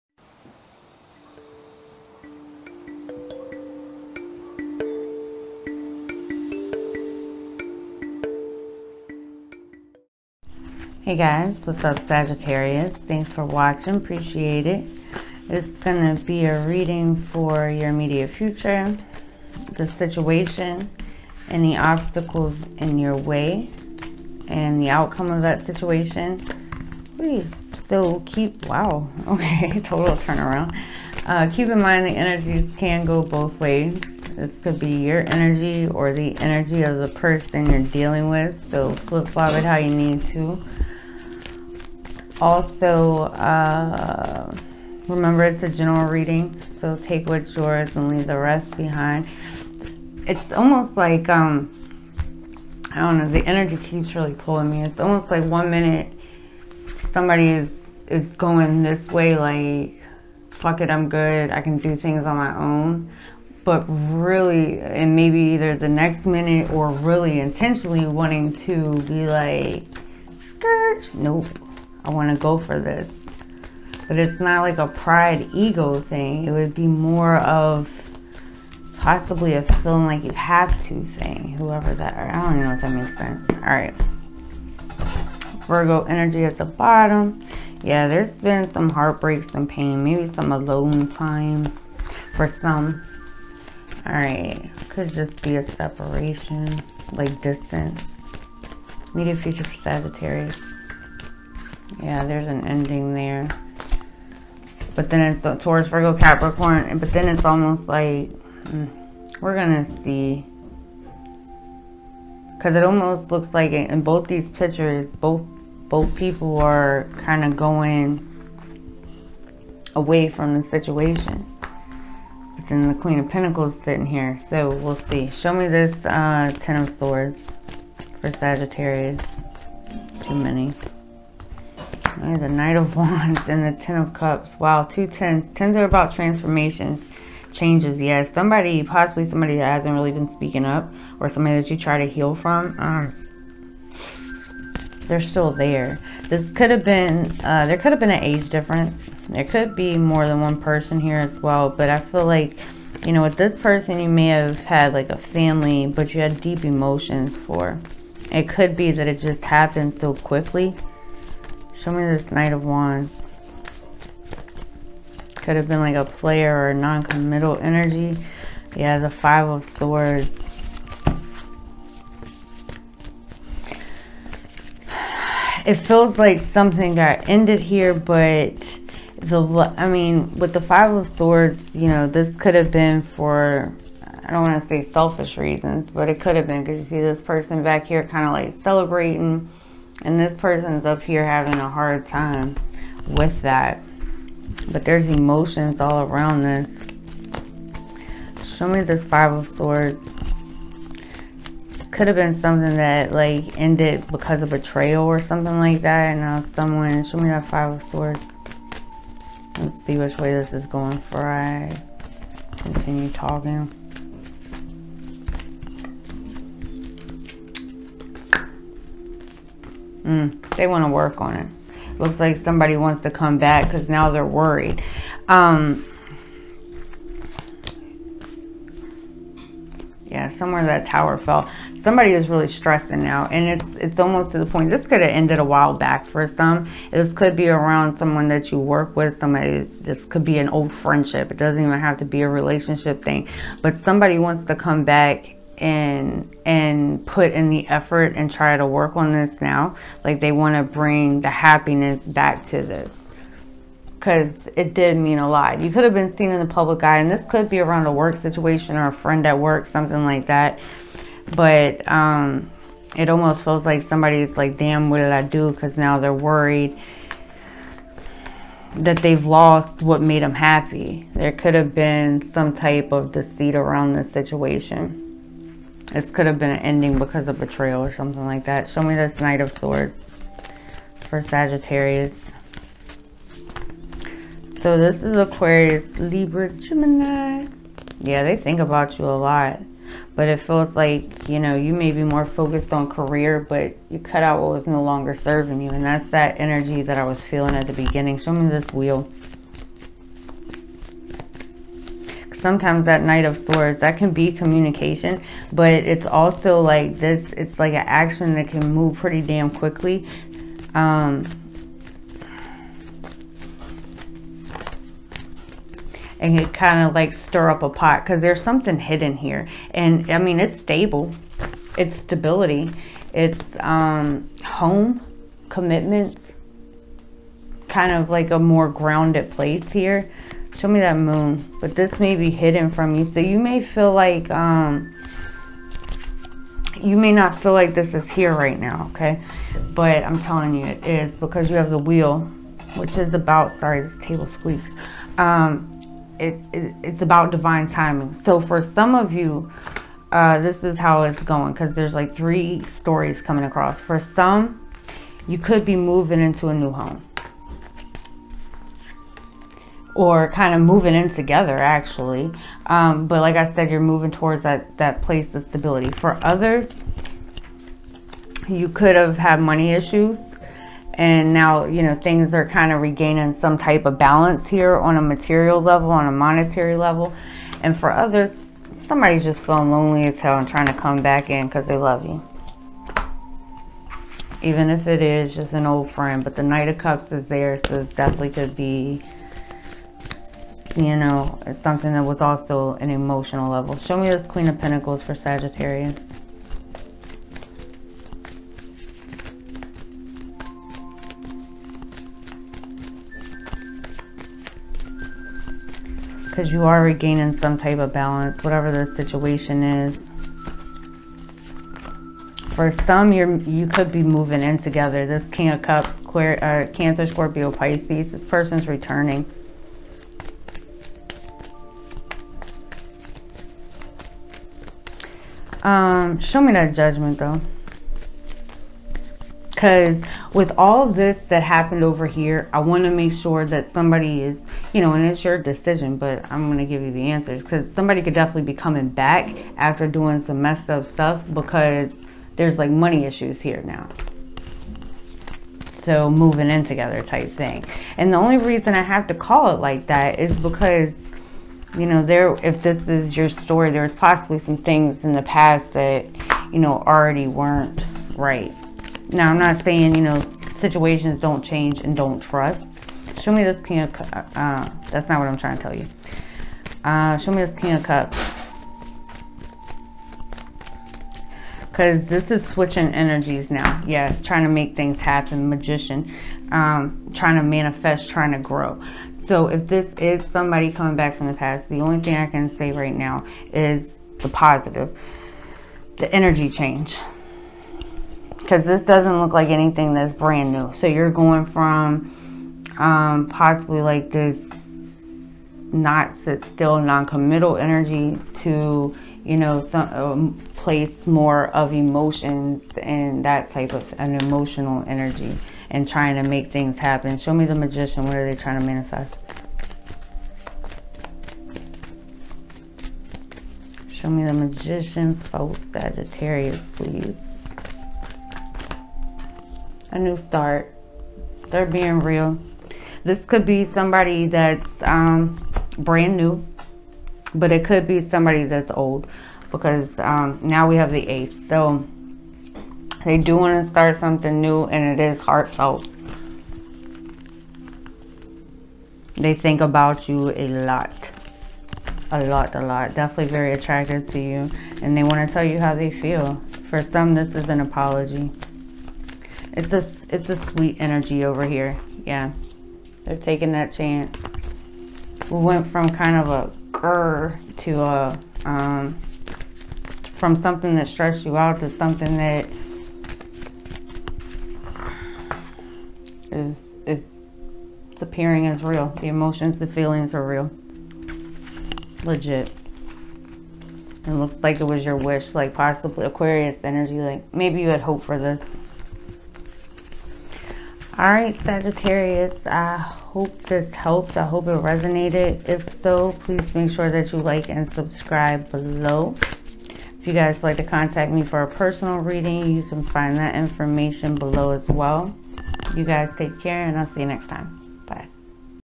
Sagittarius Tarot...Amazing Outcome...This Person Truly Loves You....Immediate Future
I apologize for the stactic caused by the microphone.  I am working to fix it asap.

Decks used:
Gilded Tarot: Ciro Marchetti
Everyday Tarot
The Light Seer's: Chris-Anne
Gilded Tarot Royale:  Ciro Marchetti/ Barbara Moore
Mermaid Tarot: Leeza Robertson
Angel Answer Oracle:  Radleigh Valentine